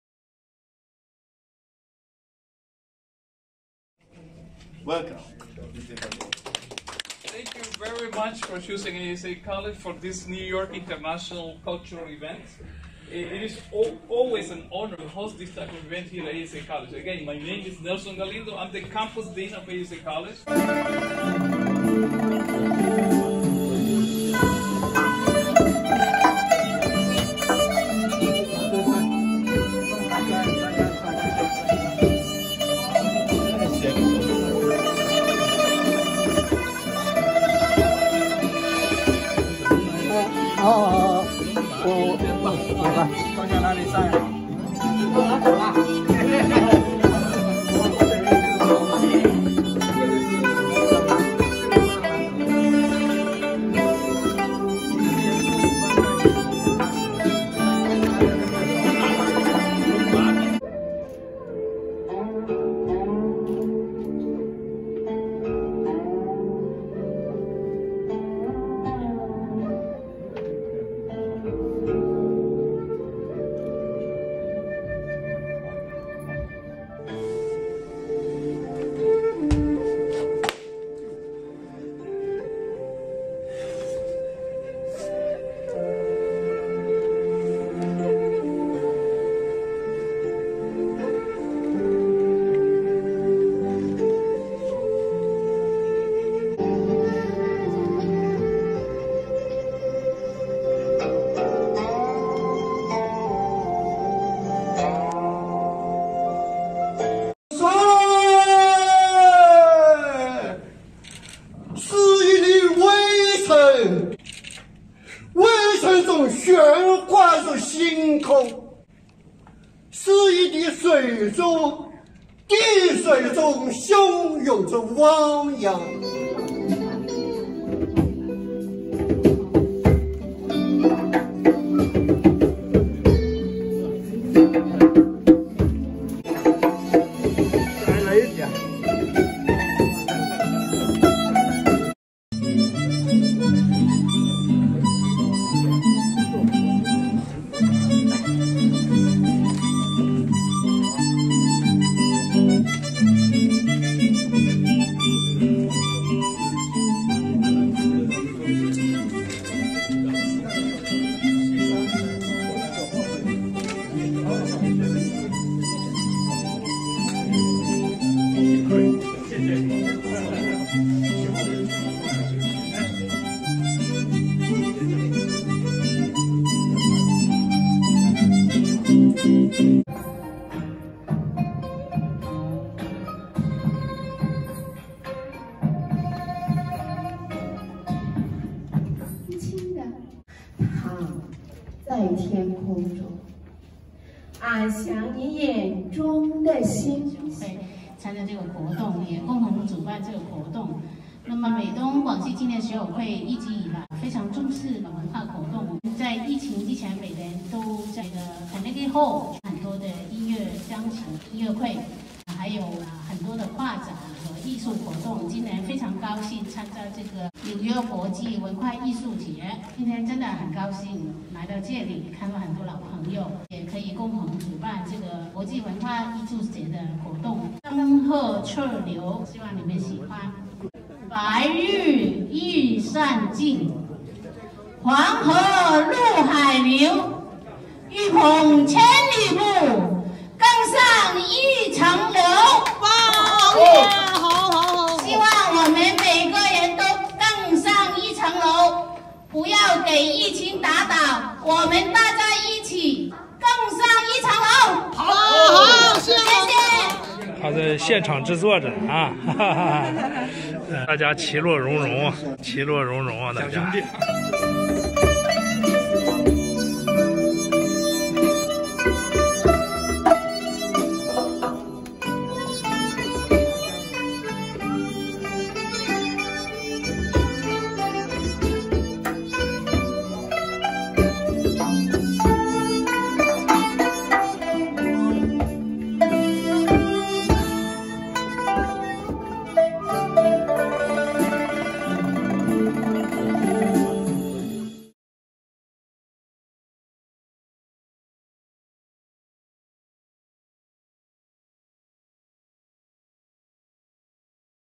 New York International Culture and Arts Festival 2022 紐約國際文化藝術節
夕阳学习与分享，今期分享、精彩无限、
紐約國際文化藝術節

        每年5月是美國“亞太傳統文化月”，為了慶祝和紀念這個特殊的月份，紐約ASA大學聯合社區數家文化藝術社團共同主辦並推出豐富多彩的文化藝術表演系列活動。

       文化藝術節活動會在ASA大學曼哈頓校園和法拉盛市政廳拉開序幕。紐約國際文化藝術節系列活動會從2022 年4月13日持續到11月2日。

      活動安排如下：

        4月13日
       新聞發布會、文學朗誦會、書法筆會雅集
       地址：ASA大學曼哈頓校園

         4月23日—5 月1 日
      紐約藝術家迎春聯展及表演
        地址：Flushing Town Hall

        5月18日
      國際茶日文化藝術節

        5 月開始，每月最後一周的周三下午2-5點
      聯合國和平音樂會
        105 W33 Street，New York，NY 10001 （在6 -7 大道之間）
       交通：B、D、F、M、R、N、Q、W、A、C、E、1、2、3、LIRR在曼哈頓34 街下車

        紐約國際文化藝術節籌備委員會計畫在ASA大學曼哈頓校園推出藝術群展、藝術個展、音樂舞蹈綜合演出專場、美術學校師生群展、現場壁畫創作、現場書法和繪畫表演、傳統手工藝品展示、武術表演與太極養生禪、茶道與花道表演、書展、讀書會、文學討論會及朗誦會、書法筆會雅集、沙龍音樂會、茶會雅集……

        ASA College 在新冠疫情前長期聯合社區眾文化藝術社團每年舉辦系列中國文化月、歡慶亞太裔傳統月文化藝術節、東方遇見西方文化藝術節系列活動，讓來賓深刻感受東方文化和多元文化之美，為繁榮校園文化和繁榮社區文化起到非常好的作用。

       本次紐約國際藝術節的共同主辦機構如下（排名不分先後）：ASA College，CAFC美國華仁協會，中國書畫名家網海外部，美國紐約皇后畫院，梁坤鈿舞蹈中心，世界文化藝術聯合會，紐約藝能畫院，美東廣西青年學友會，美國九龍藝術研究院，Neoillusion Teaism,  Red Square 紅四方，國際華嚴會，美國心武門拳社，美國UNAA ART藝術教育，陽光鋼琴學校，聯合國網合集團，美中學術交流與翻譯中心，Huang Xiang Culture Studio & KunLun  Press。

        表演藝術家名單如下（排名不分先後）：
        舞蹈家梁坤鈿、武術家楊龍飛、鋼琴家董仁鵬、朗誦家董濤、茶道家紅四方、茶藝家魏峰青、插花師張怡、打擊樂演奏家William Ruiz、琵琶演奏家張宇愽、古箏演奏家劉群等等，隨著活動進行，還將會有更多的藝術家和表演者加入我們的行列。

       五月亞太裔傳統月，紐約國際文化藝術節將以藝術展覽、藝術表演、文學討論會、圖書展、詩歌朗誦會等形式表達對亞裔和亞太裔美國人的支持和表彰。任何針對亞洲人、亞裔美國人和太平洋島民的仇恨犯罪、暴力和言語攻擊都痛苦地提醒著人們，種族主義、偏執和仇外心理是美國面臨的長期挑戰。在新冠疫情使得美國歧視亞裔情緒個案激增的當下， 如此的文化活動目的在於召喚社會、人性的反思，來避免歷史上黑暗的重演。抗擊歧視和仇恨還需要亞裔人群更多的與其他族裔團結，積極參與文化及各種社會活動，親密傳統的優秀、共創和諧的美麗、喚醒腐敗的道德、維護人類的公義、多元化地繁榮社區，並以此提升社會文明。

紐約國際文化藝術節籌備委員會